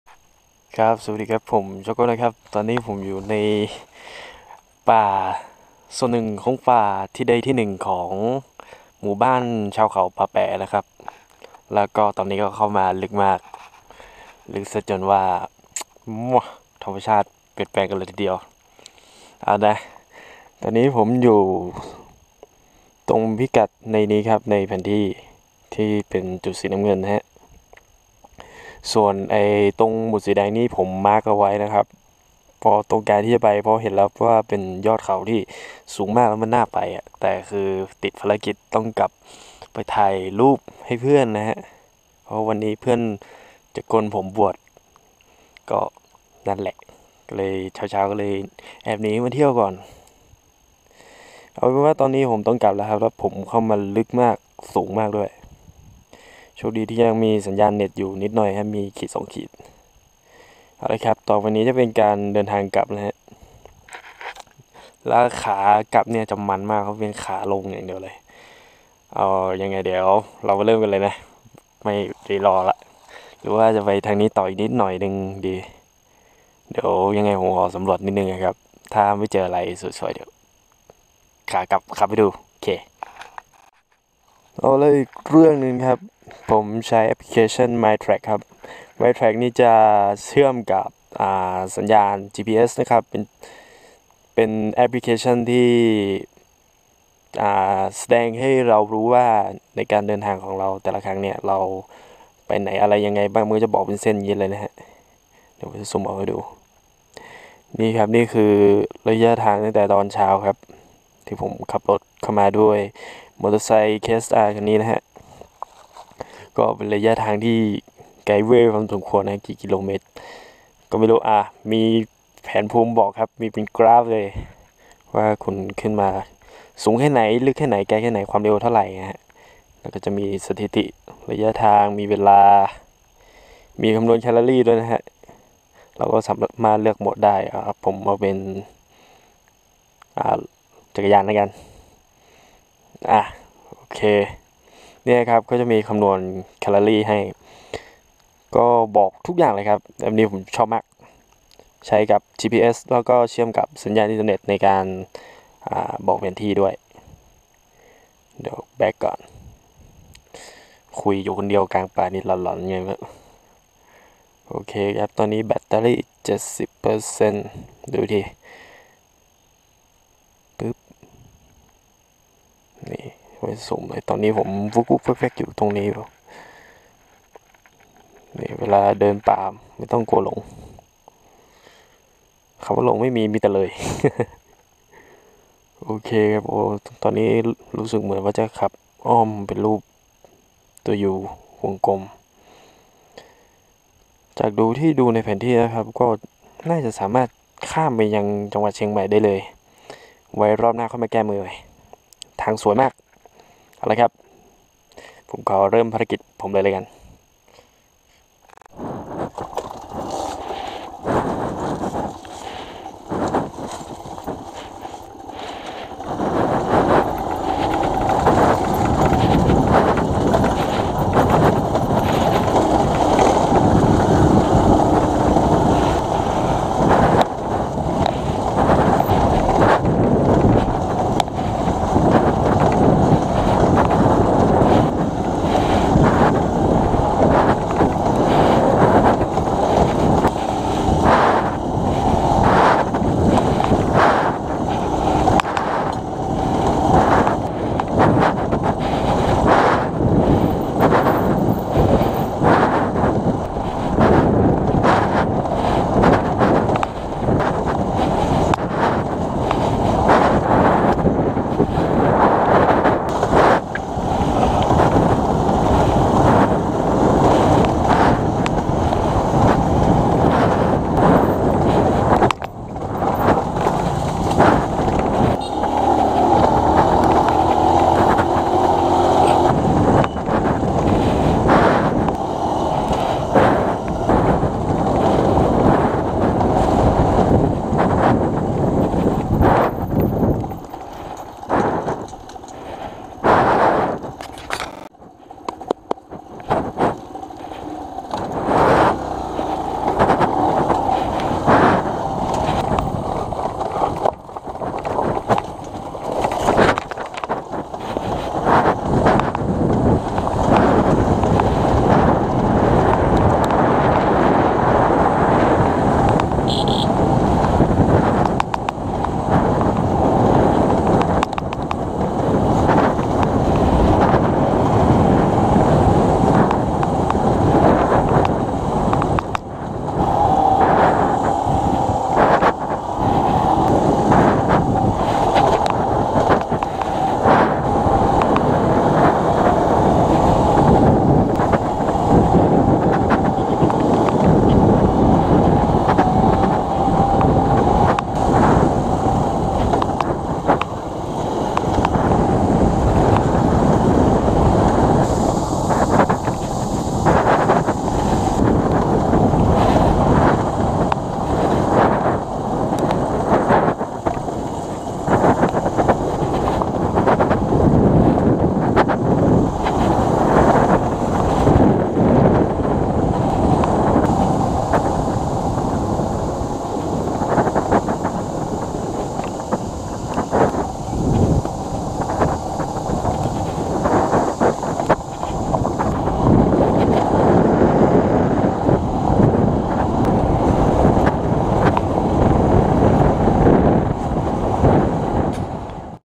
ดอย ป่าแป๋ อ.บ้านโฮ่ง จ.ลำพูน Kawasaki KSR [HD 720p 48fps]
ดอยป่าแป๋ เป็น ดอยที่ที่มีความสูงชันแห่งหนึ่งใน อ.บ้านโฮ่ง จ.ลำพูน ครับ
 โดย ใช้ทางหลวงหมายเลข 106 มุ่งหน้าไปยังค่ายลูกเสือ หมู่บ้านวังหลวง หรือทางเข้าน้ำตกวังหลวง ไล่จากระความสูงชันไปเรื่อย จากป่าเต็งรัง ไปจึงถึงป่าดิบชื้น ช่วงแรกเป็นทางคอนกรีต และทางดินไปเรื่อจนถึงหมู่บ้านครับ